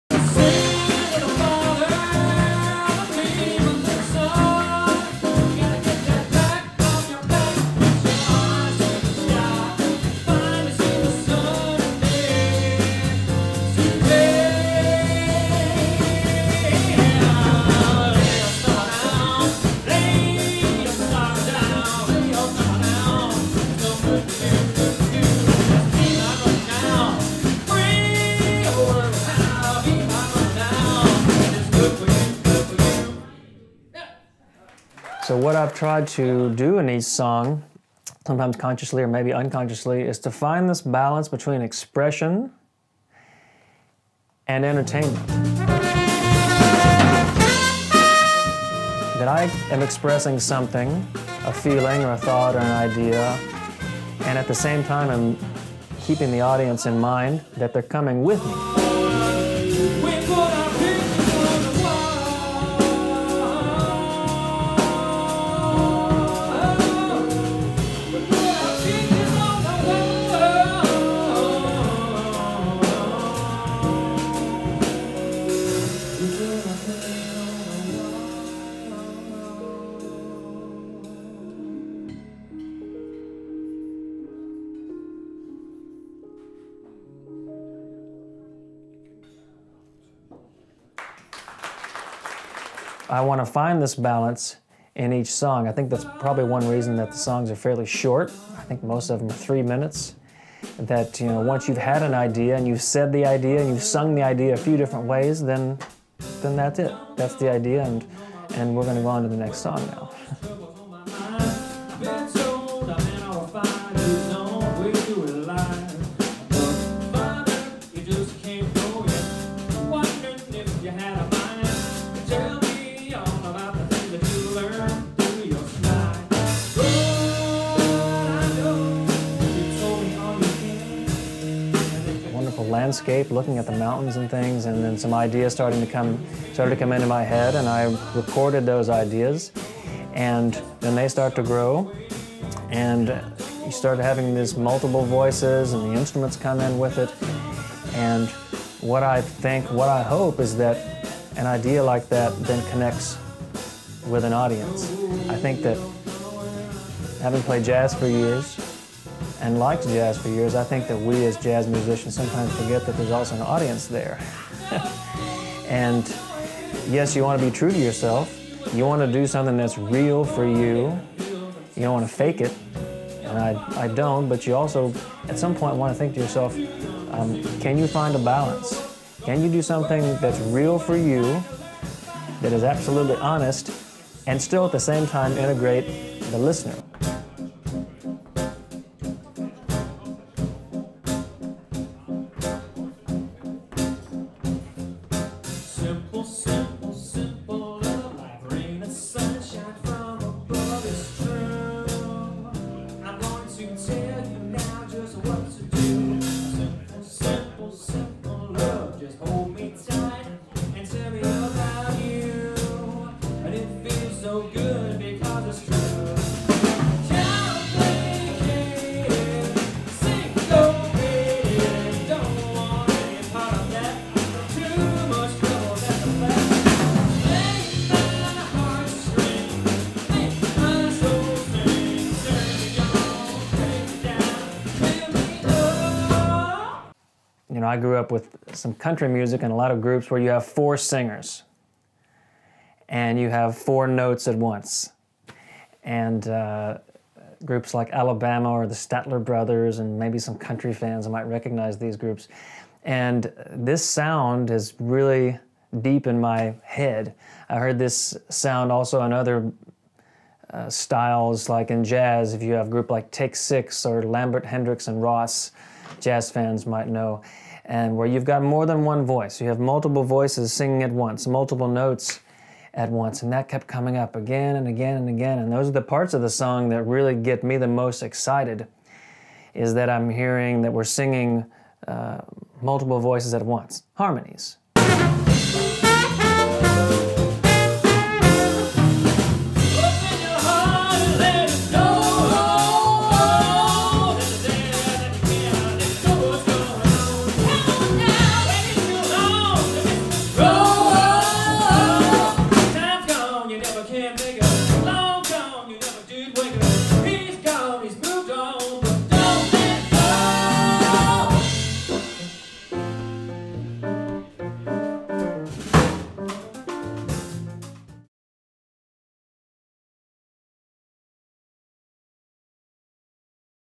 Darrell Wyatt And Tennessee Soul
Darrell Wyatt introduces his band: Darrell Wyatt And Tennessee Soul.